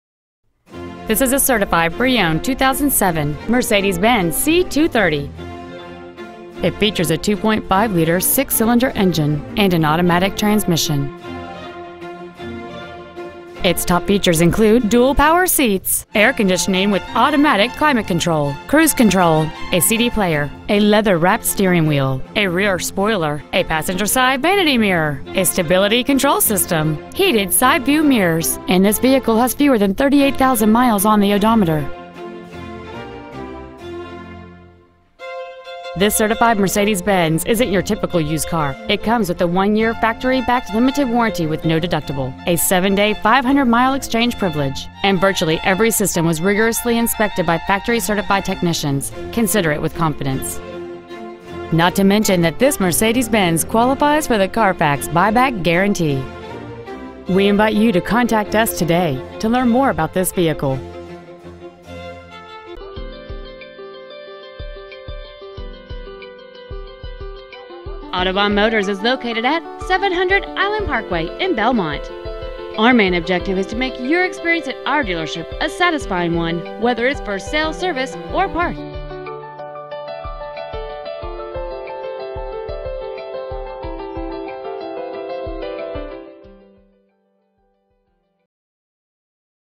Certified 2007 Mercedes-Benz C230 Sport Belmont CA 94002
Year : 2007

 Make : Mercedes-Benz

 Model : C230

 Engine : 2.5L V-6 cyl

 Trans . : Automatic

 Exterior : Black

 Miles : 37,203

 Interior : Black

 Autobahn Motors

 700 Island Parkway

 2007 Mercedes-Benz C230 Sport Belmont CA